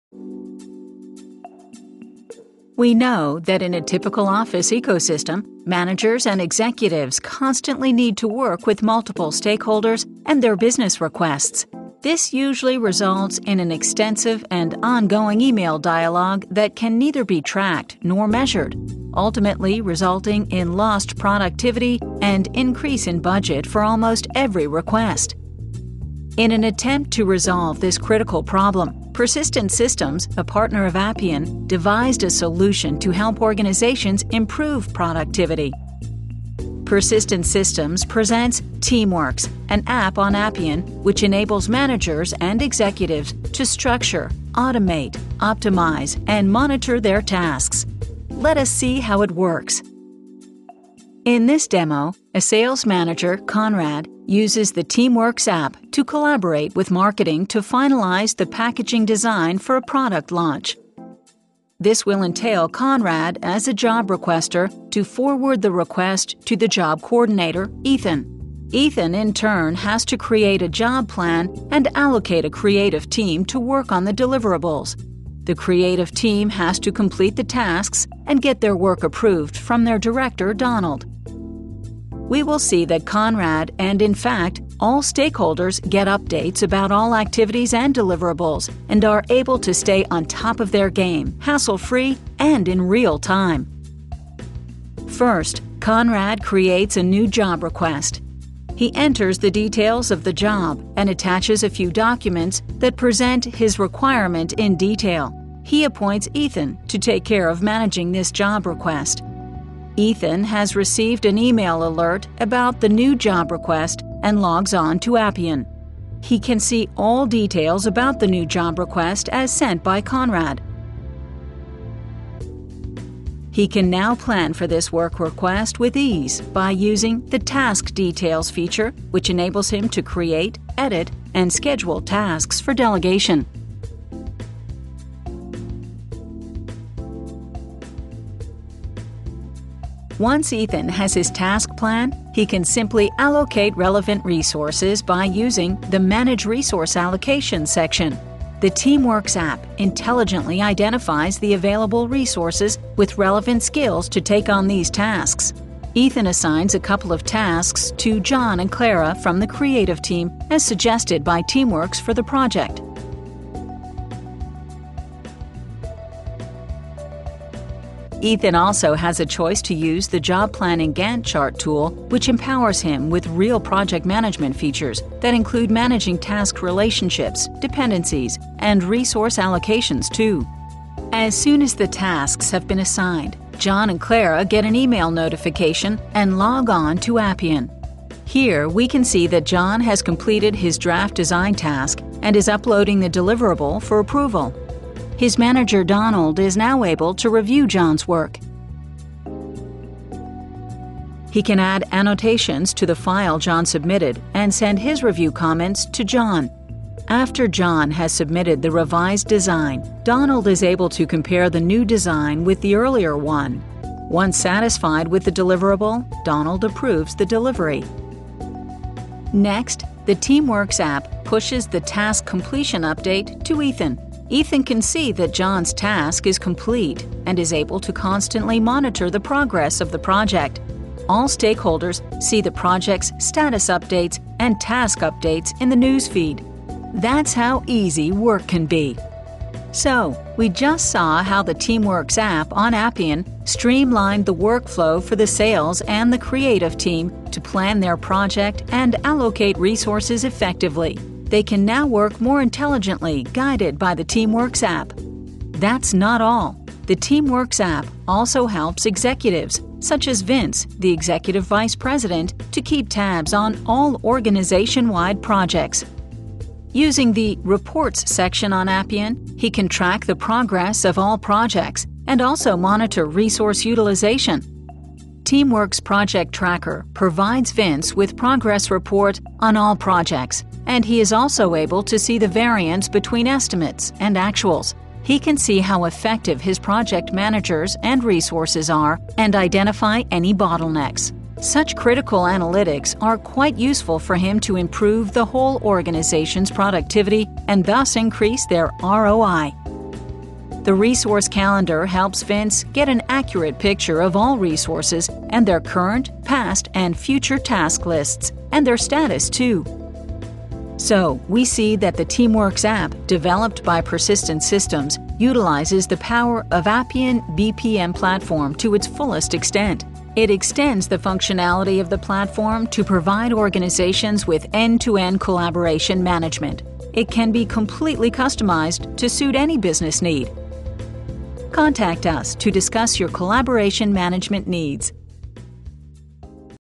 TeamworxApp for Appian
Watch how the Teamworx app enables managers and executives to structure, automate, optimize and monitor their tasks.